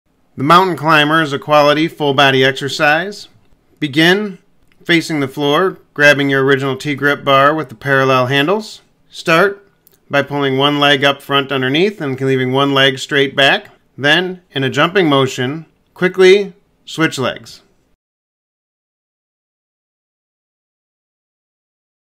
Mountain Climber Exercise using the Original TGrip Bar
This is an excellent full-body and core exercise. The TGrip bar keeps your hands in a neutral position, allowing you to focus on proper exercise technique.